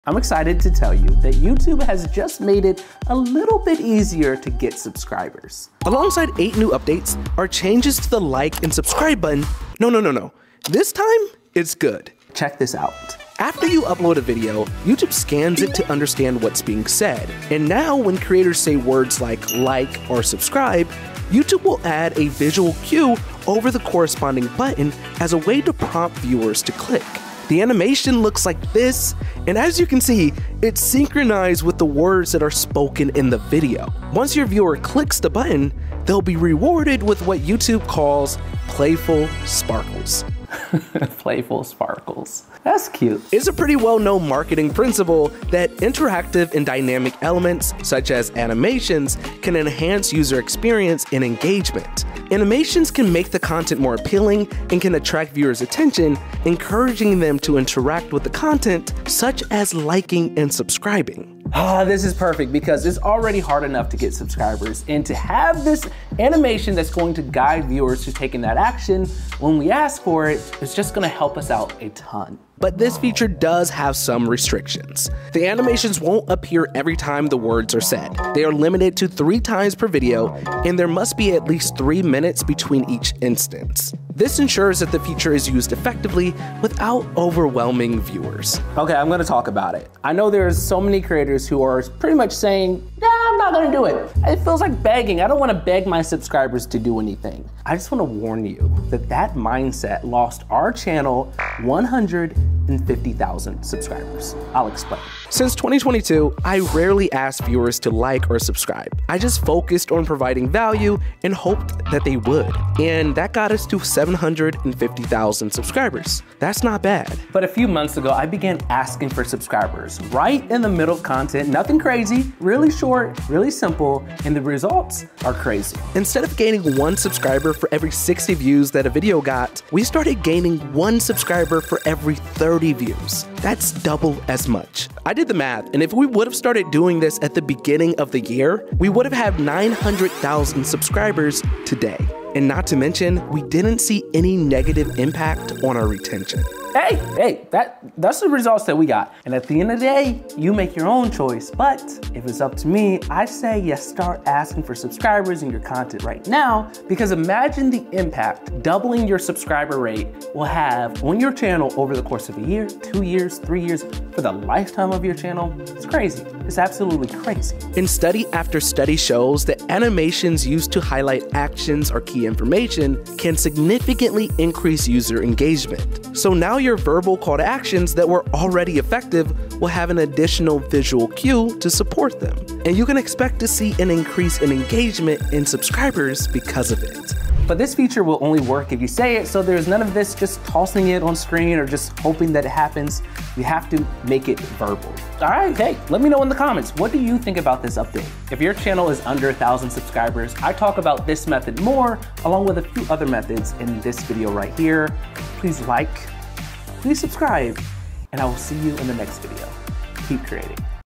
YouTube's New Subscribe Feature Is Awesome!
🔔 BIG UPDATE! YouTube has revamped the way we gain subscribers, and it's all for the better! 🚀

📌 Key Takeaways:

This animation is synchronized with the words and rewards viewers with "playful sparkles" when they interact.
The power of animations in marketing is undeniable, and it can boost user engagement significantly.

🧐 Delving Deeper:

This update isn't unlimited! Animations are restricted to three times per video with a 3-minute gap in between.
Our tests show we could double subscriber rates by simply asking.
It's ultimately your choice to use this feature, but the potential benefits are massive.

🔍 Additional Insights:

Studies have shown that animations highlighting actions can majorly bump up user engagement.
Verbal calls to action, now coupled with a visual cue, can amplify your subscriber gains.

📣 To the Growing Creators:
If you're still under the 1K subscriber mark, check out my deeper dive into this method and other strategies to boost your subscriber count.

❤️ Sending love to this amazing community. I'd love to hear your thoughts on this update.

00:00 - Introduction to YouTube's New Feature
00:17 - How the New Feature Works
00:47 - Importance of Interactive Elements
01:16 - Restrictions of the New Feature
01:32 - The Impact of Asking for Subscribers
02:51 - The Power of Visual Cues
03:11 - The Importance of Verbal Calls to Action
03:19 - Conclusion and Call to Action